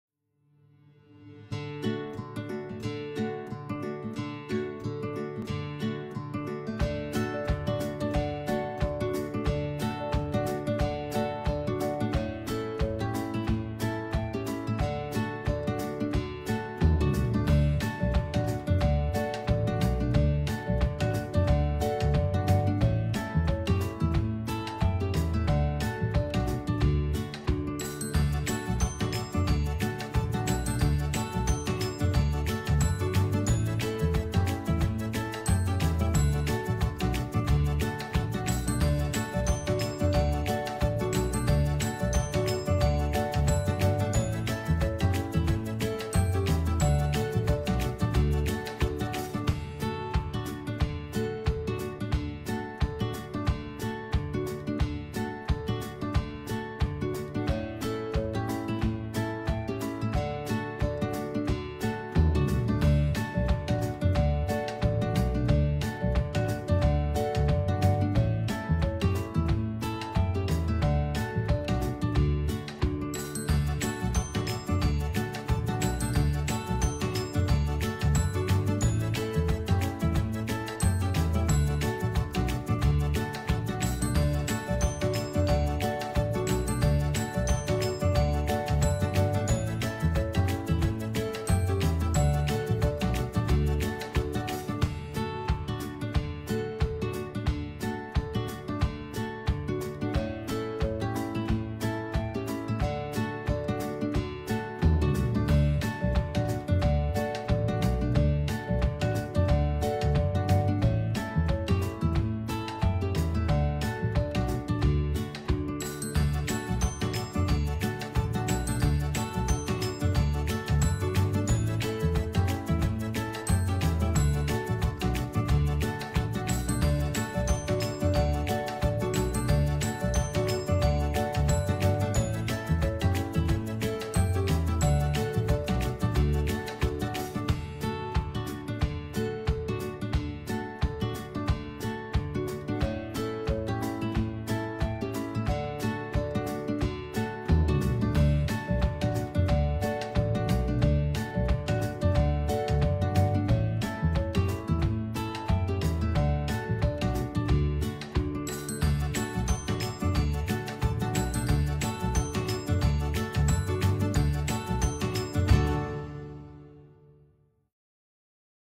Where in the world are the Aitken Artists?
Here is a video of our Aitken artists on a virtual field trip!